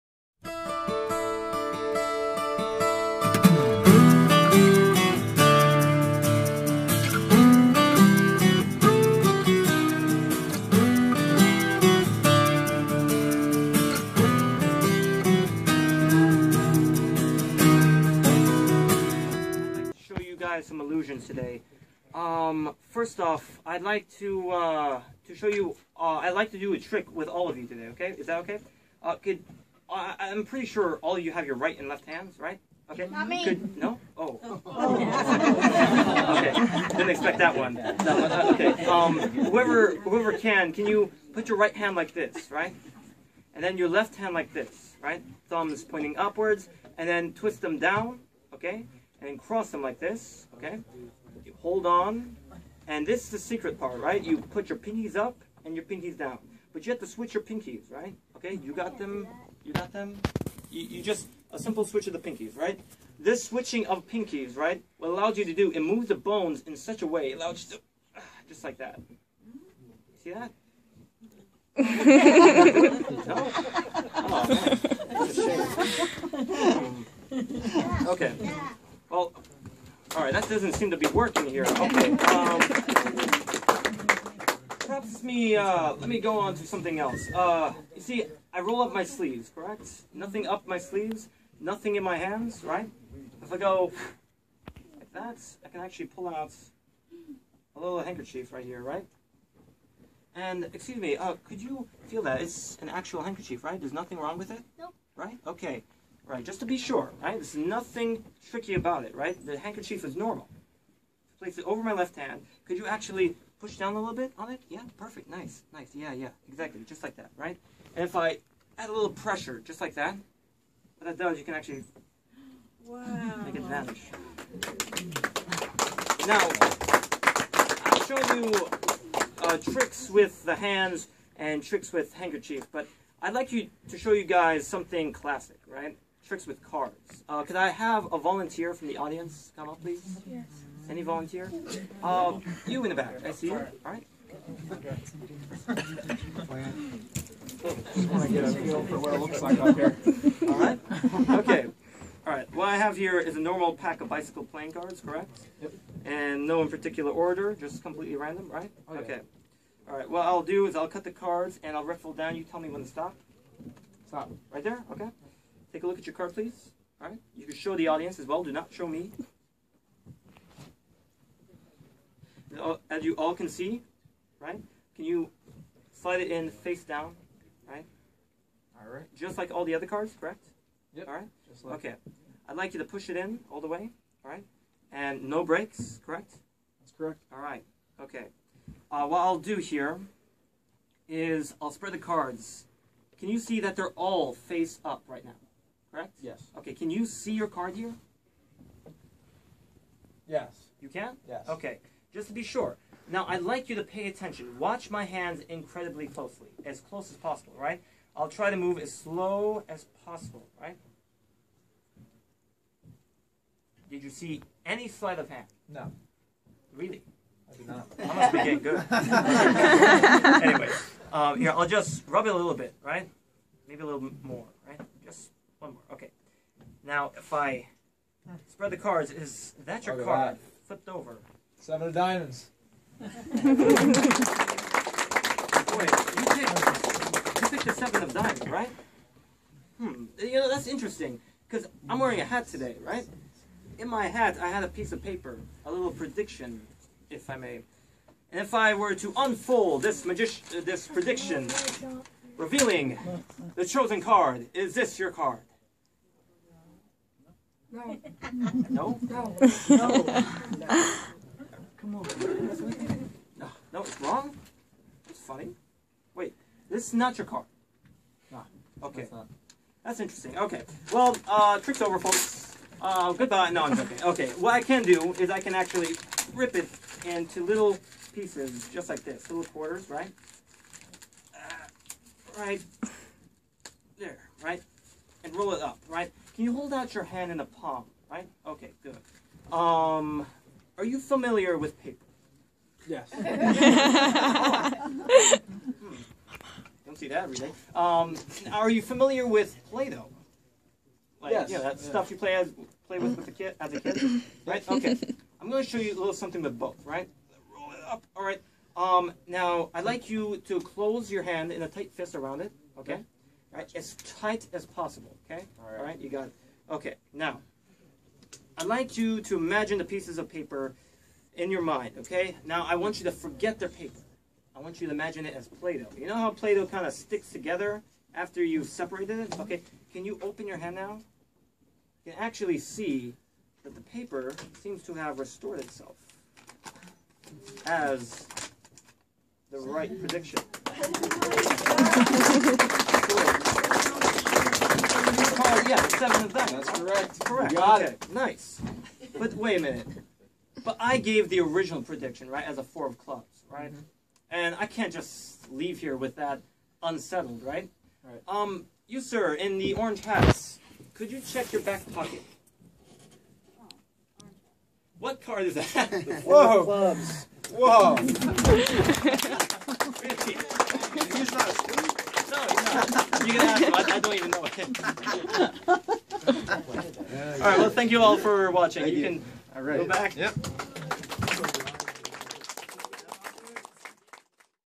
Secret Ace: Magic show!
Yesterday I was performing on stage at a Mingus Magic. A magic shop in Reading, Pennsylvania. On Halloween they have something called magic till midnight. Which is just a lot of magicians performing.